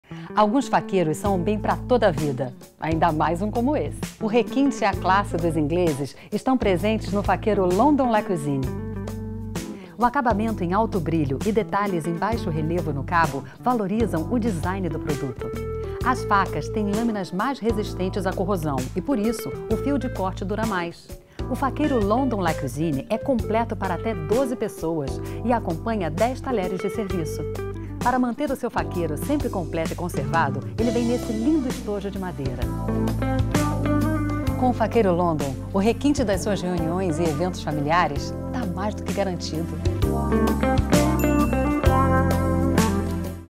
Faqueiro London La Cuisine | Shoptime
Muito mais sofisticação em suas refeições! O Faqueiro London conta com 130 peças em aço inox que servem até 12 pessoas.